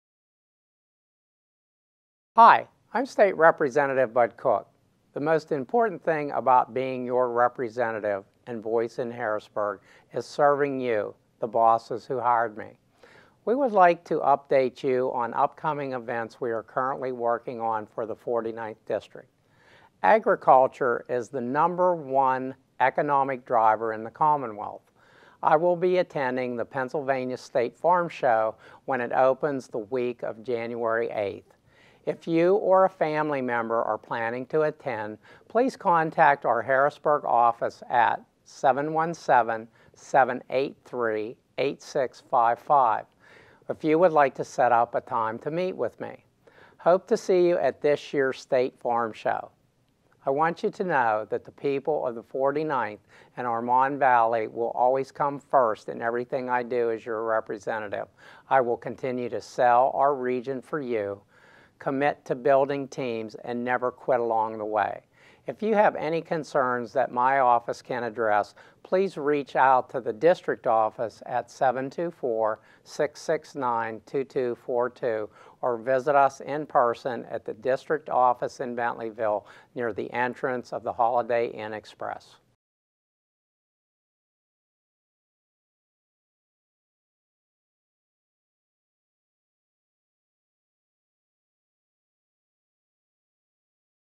Mon Valley Update 1-3-18 (Pa. Farm Show)
In this Mon Valley Update, Pa. State Rep. Bud Cook invites constituents to the coming Pennsylvania Farm Show.